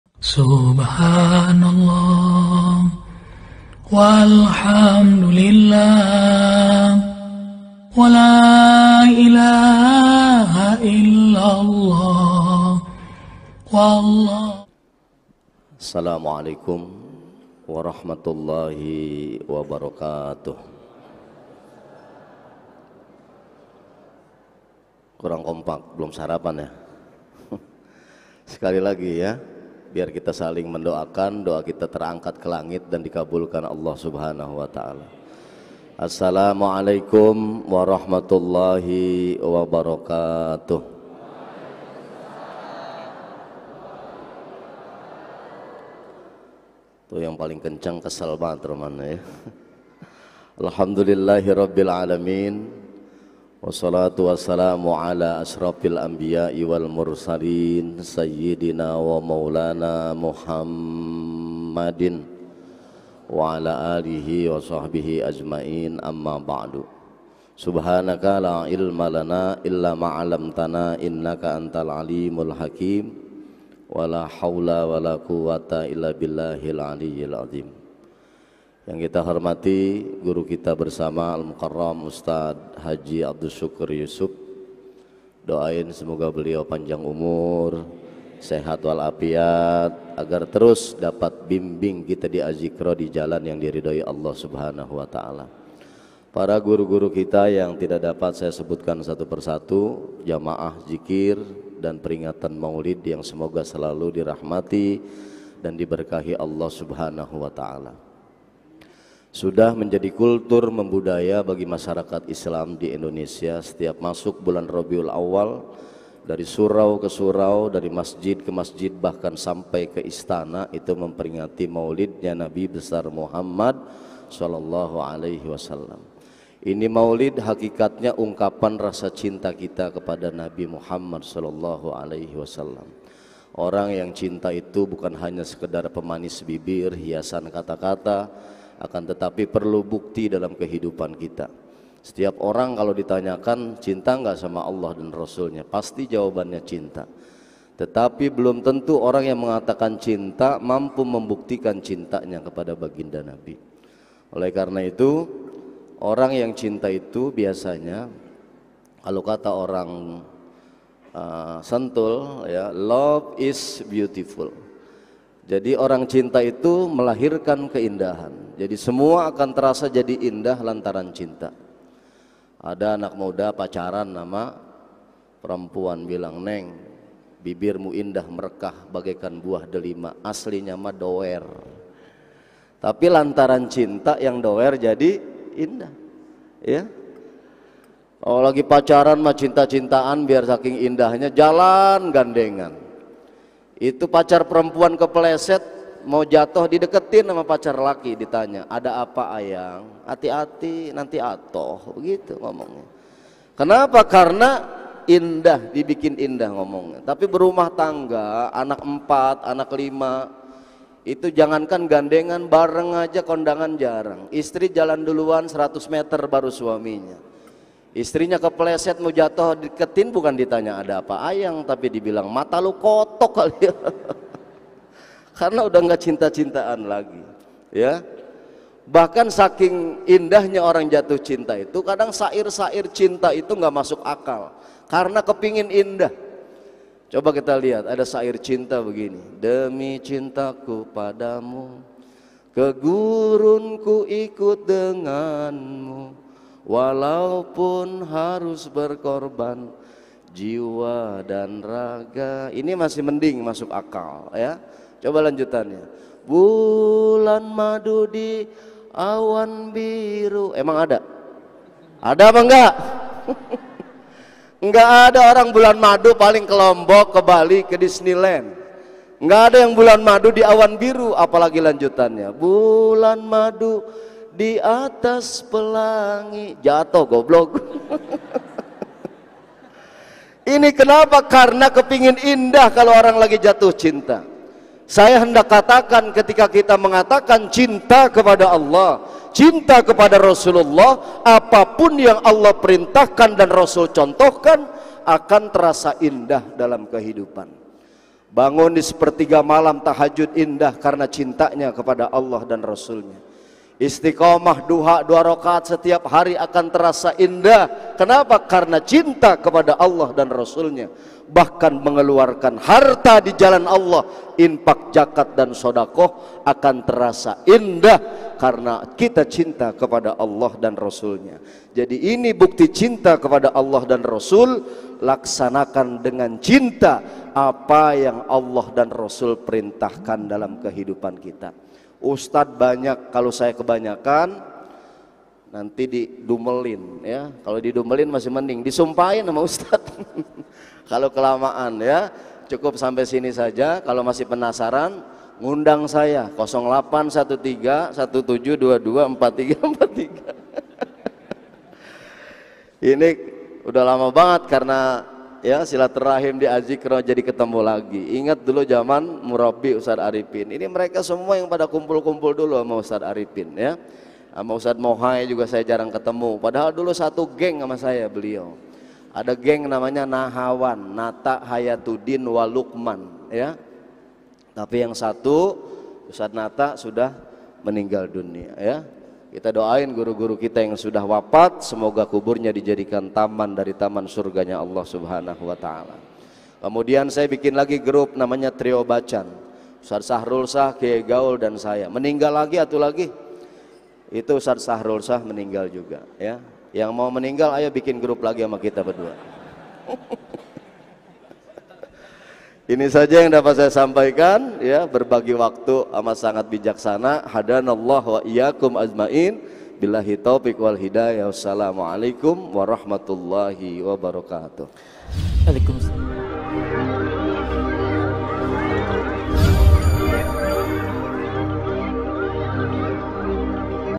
Jangan Salah Meletakkan Cinta Selain Kepada Allah Dan Rasulullah - Ustadz Lukman Sayadi M.Ag
Official Media Masjid Az-Zikra Sentul, Bogor

Donasi Support Az-Zikra Media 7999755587 Rekening BSI an DKM AzZikra